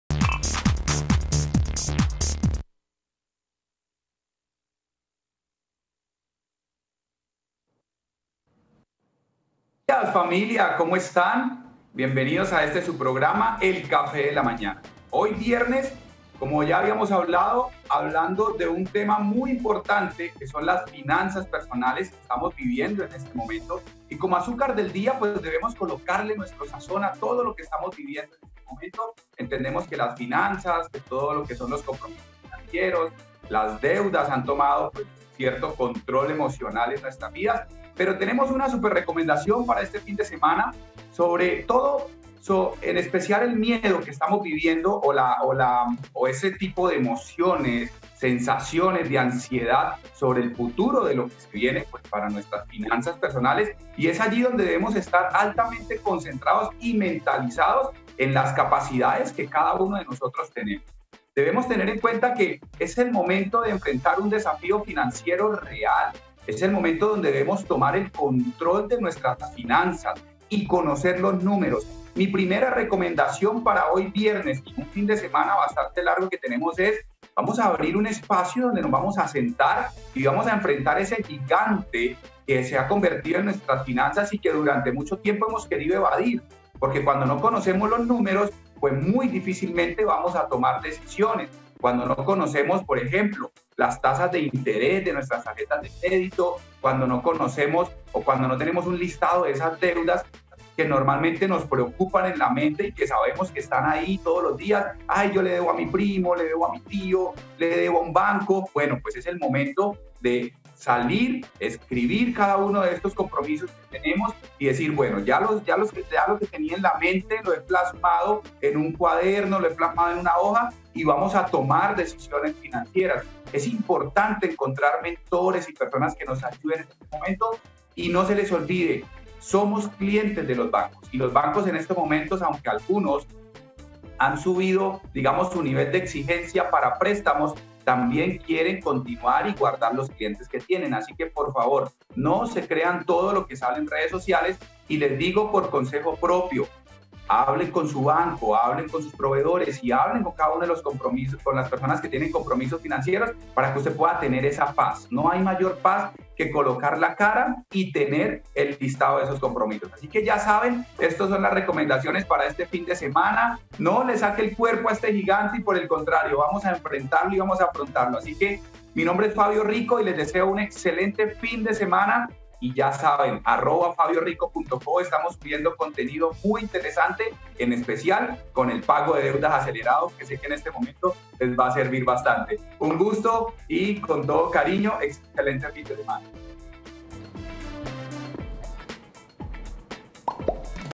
2020 el año de los desafíos financieros
Es momento de estar concentrados en nuestras finanzas personales, el 2020 se convirtió no solo en un año de pandemia, sino además en un pasaje especial para repensar nuestros suministros económicos y las deudas que tenemos con entidades bancarias o incluso con amigos, este es el instante ideal para afrontar esta situación y hablar con ellos.